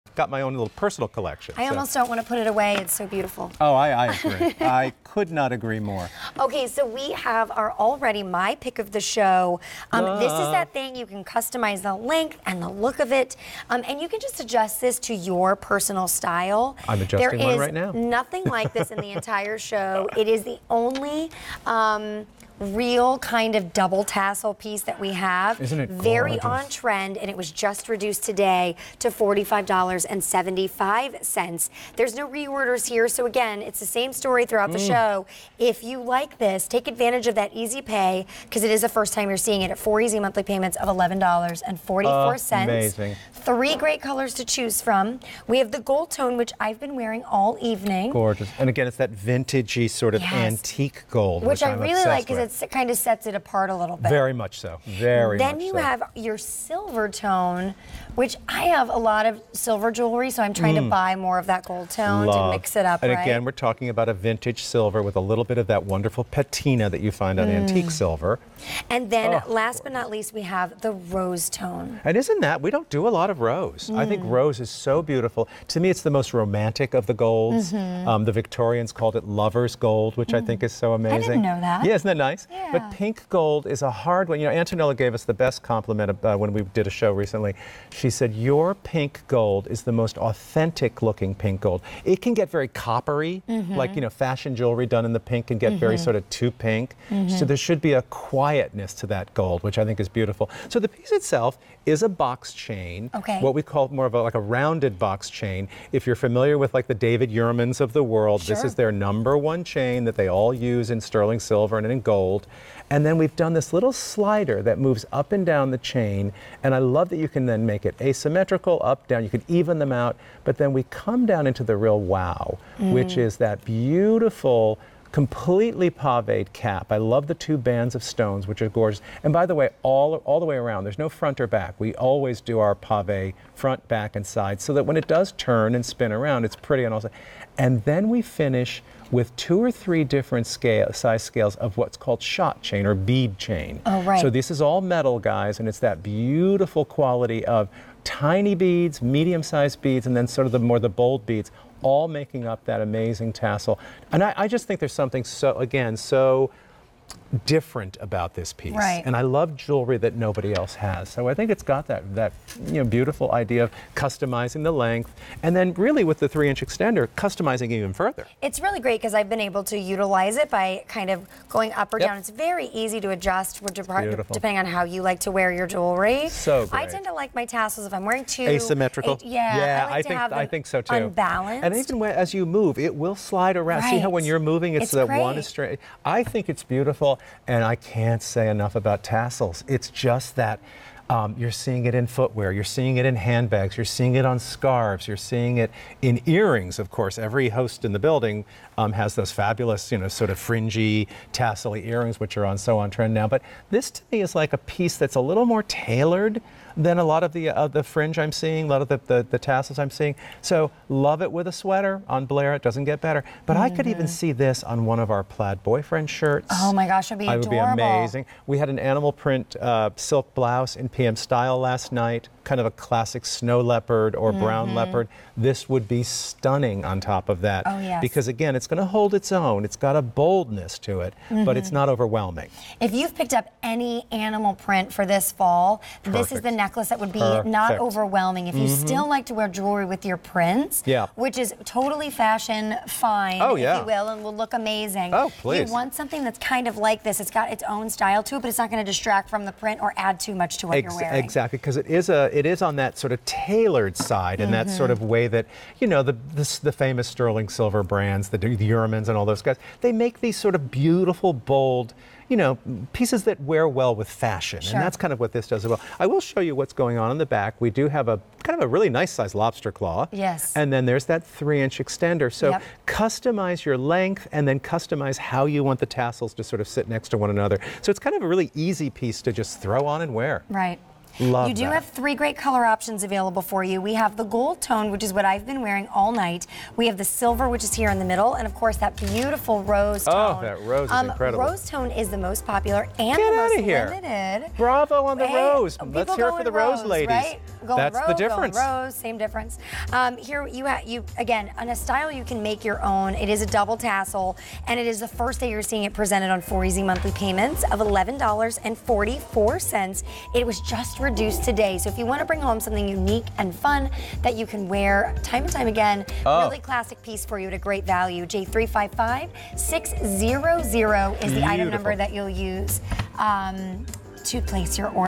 Joan Rivers Sliding Chain 31" Necklace with Double Tassel on QVC
Joan Rivers Sliding Chain 31" Necklace with Double Tassel

Wear it low or high--you decide with this Joan Rivers sliding chain necklace. The crystal-accented bar easily adjusts to whatever neckline you're wearing, and the double tassels add fun movement. From the Joan Rivers Classics Collection&reg;.